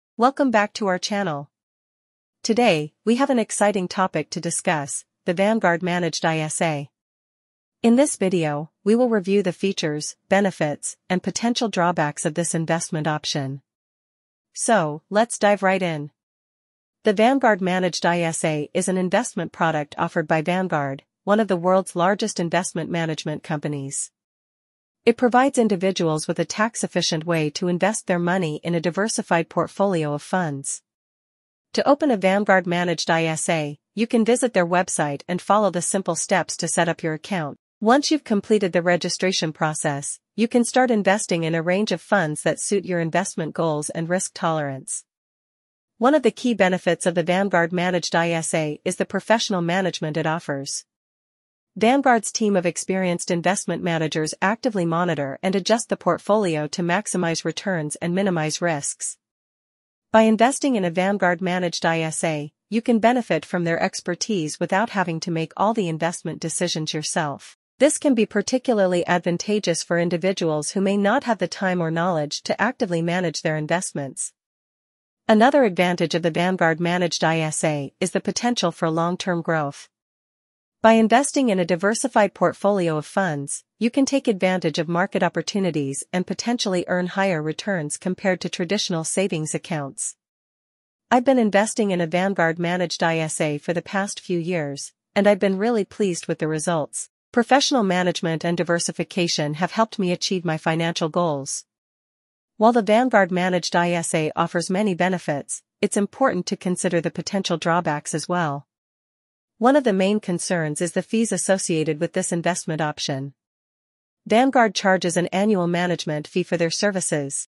Vanguard Managed ISA Review
"Navigate the world of tax-efficient investing with our in-depth Vanguard Managed ISA Review! In this video, we dissect the features and benefits of the Vanguard Managed ISA, a strategic investment solution designed to help you grow your wealth while enjoying tax advantages. Explore the managed portfolio approach that combines Vanguard's expertise with the tax-efficient structure of an ISA. From diversified asset allocation to personalized investment strategies, discover how this managed ISA can align with your financial goals. We'll break down the user experience, cost-effectiveness, and the potential for long-term wealth accumulation. Whether you're a seasoned investor or new to the investment landscape, our review provides valuable insights to empower you in making informed decisions about incorporating Vanguard Managed ISA into your financial strategy. Join us on this exploration of tax-efficient investing with Vanguard!"